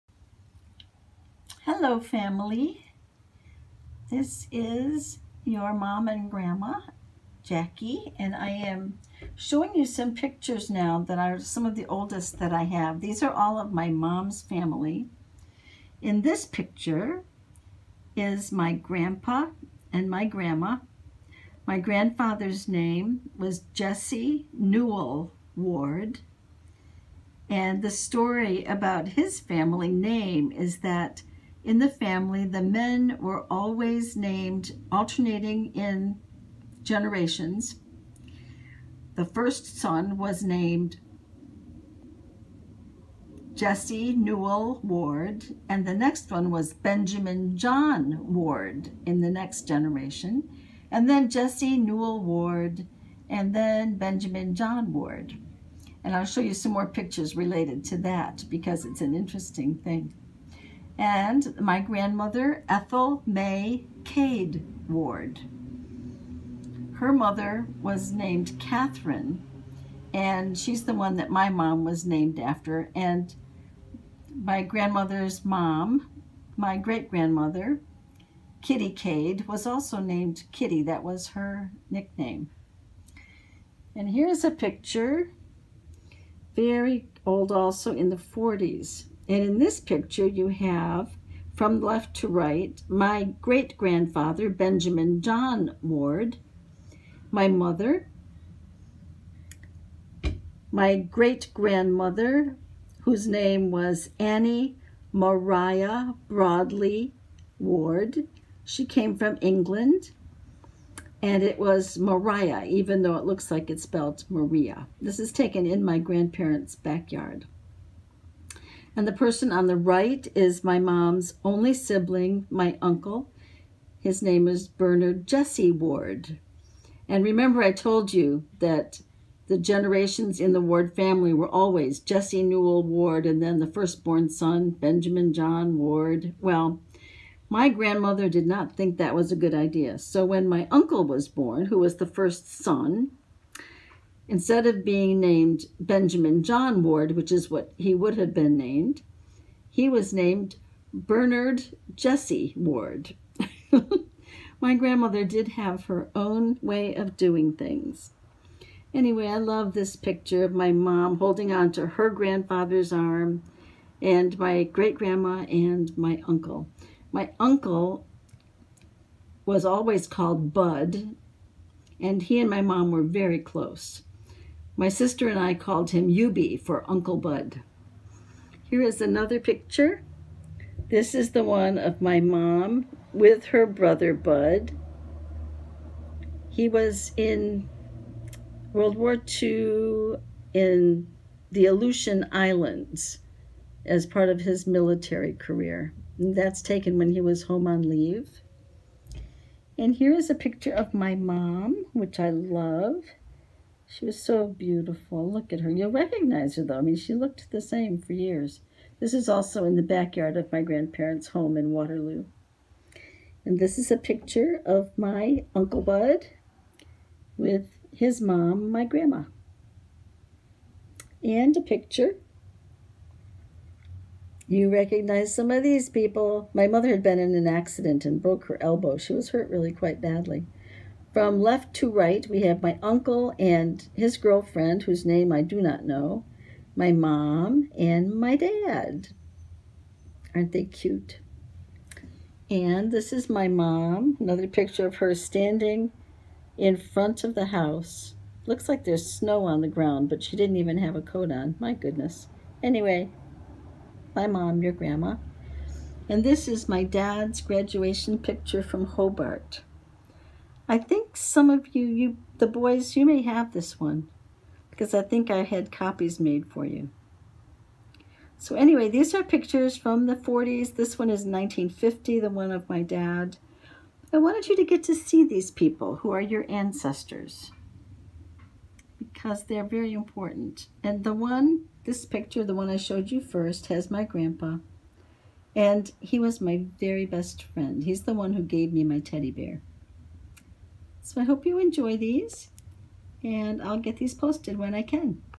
Pictures of the Ward family - 1940’s
My great-grandpa :  Benjamin John Ward
My great-grandma:  Annie Maria Broadley Ward
My grandpa:  Jesse Newell Ward
My Grandma:  Ethel May Cade Ward
My uncle:  Bernard Jesse Ward